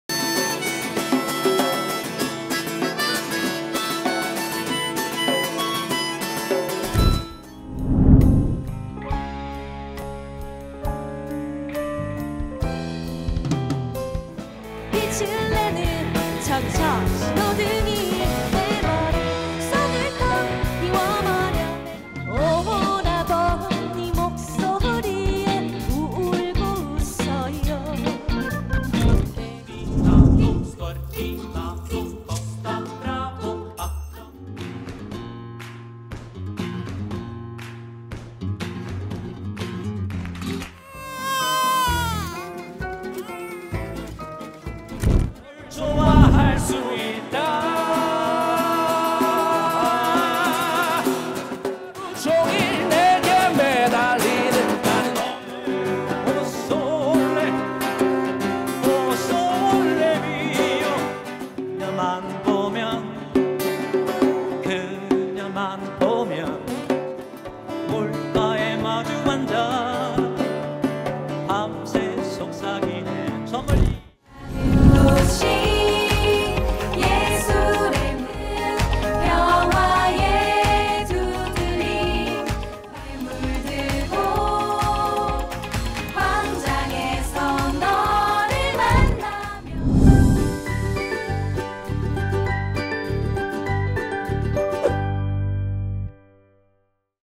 [6월 11일]2022 광주프린지페스티벌(우리동네) 2회차 하이라이트
🎊2022 광주프린지페스티벌(우리동네) 2회차 행사🎊
6월 11일 남구 주월동 푸른길공원(빅스포 뒤편) 축제 하이라이트 영상

2회차 남구 푸른길공원에서는 더 풍성해진 공연, 예술놀이·체험프로그램과
다양한 이벤트가 진행되었습니다.😍

함께해주신 공연, 체험팀과 시민여러분들 정말 감사합니다.😄

6월 18일 토요일 3회차 광주프린지페스티벌은
광주 북구 양산호수공원으로 찾아갑니다.
많은 관심과 참여 부탁드리겠습니다🤗

❗자세한 내용은 광주프린지페스티벌 홈페이지를 참고해주세요❗